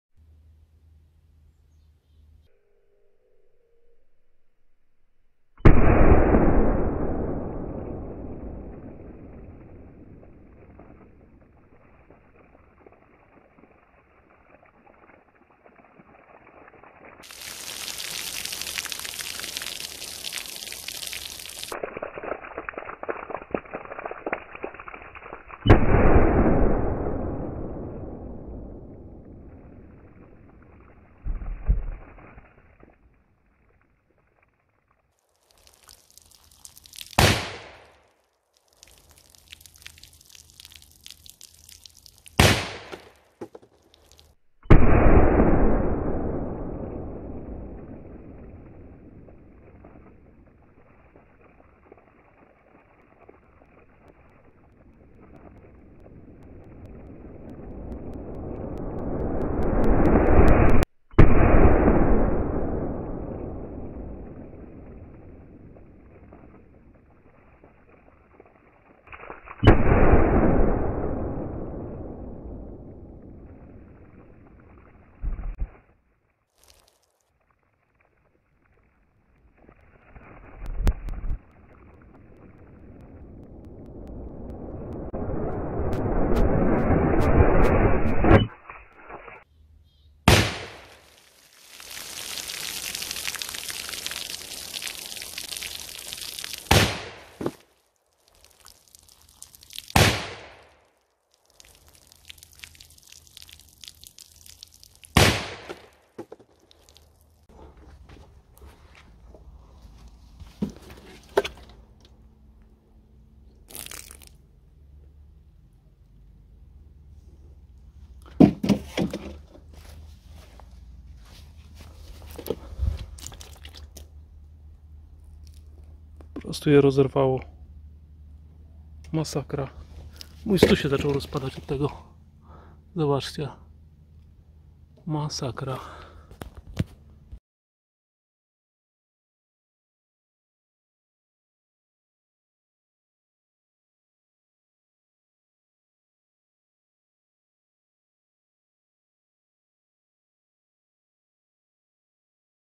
Szwajcar No.1 (FFFFg) - Olbrzymia Moc
Rewolwer: Remington 1858 - (Pietta)
Proch: Szwajcar No.1 (FFFFg) - po sam korek ~ 2.4 ml
Kule: 454 - (miękkie, dokładnie obtaczane)
Smar: Ligtning No.1
Kapiszony: RWS 1075
Szczypta przybitki ;-) w postaci kaszy manny...